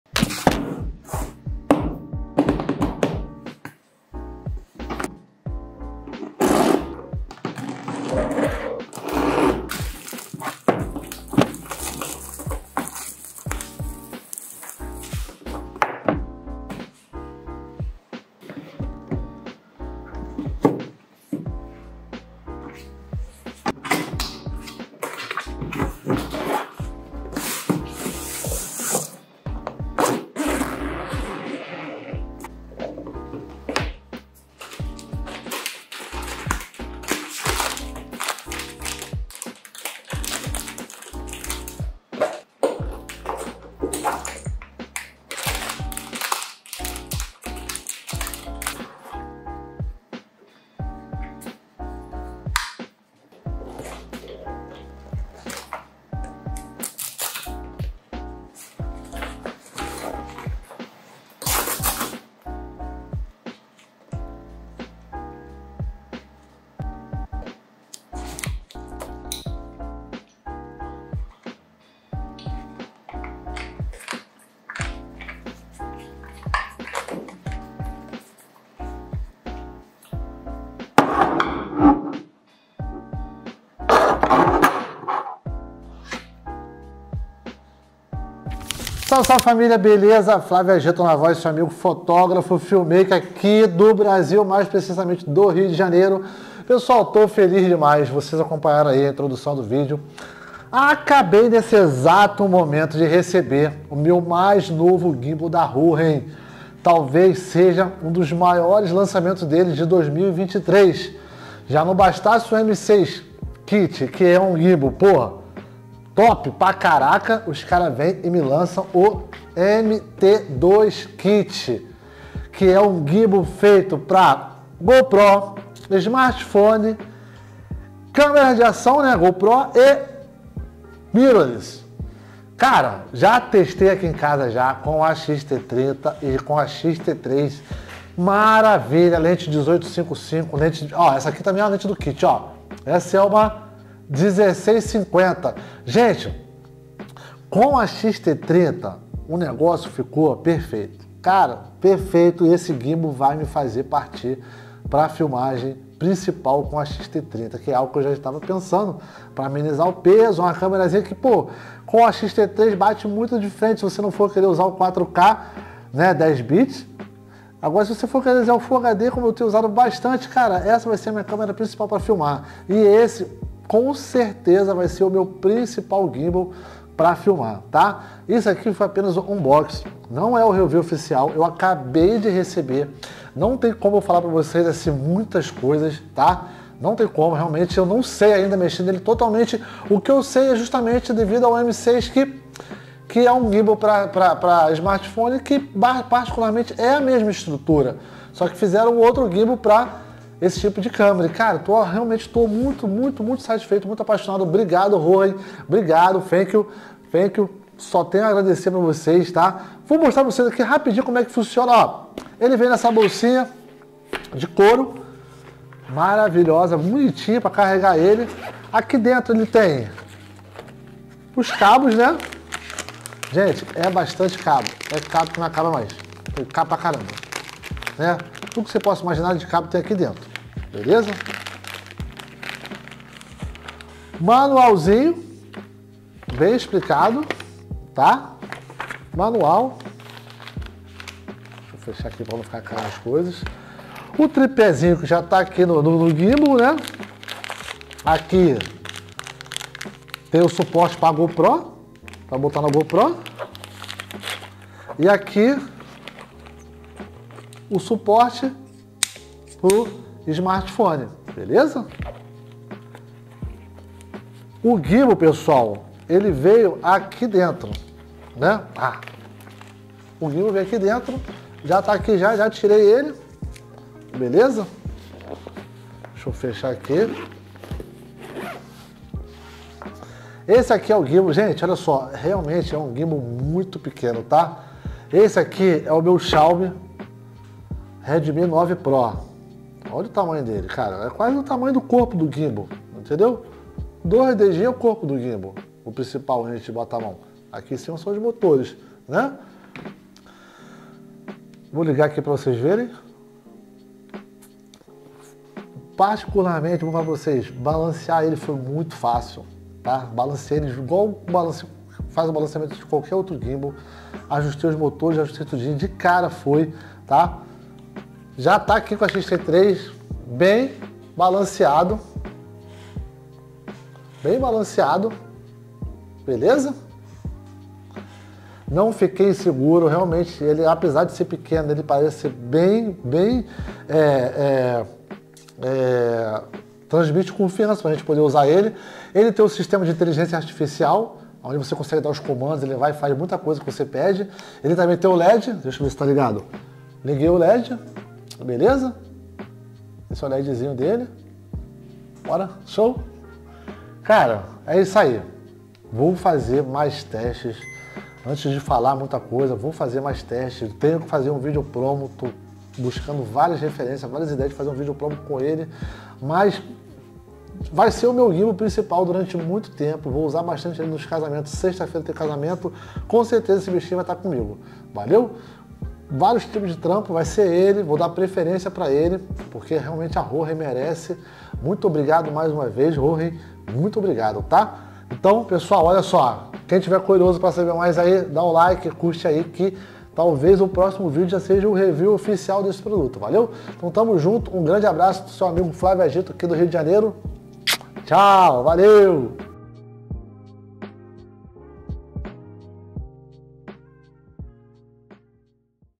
MT2 KIT GIMBAL | HOHEM | Unboxing
Unboxing desse mega gimbal que recebi exatamente na data de hoje 03/07/23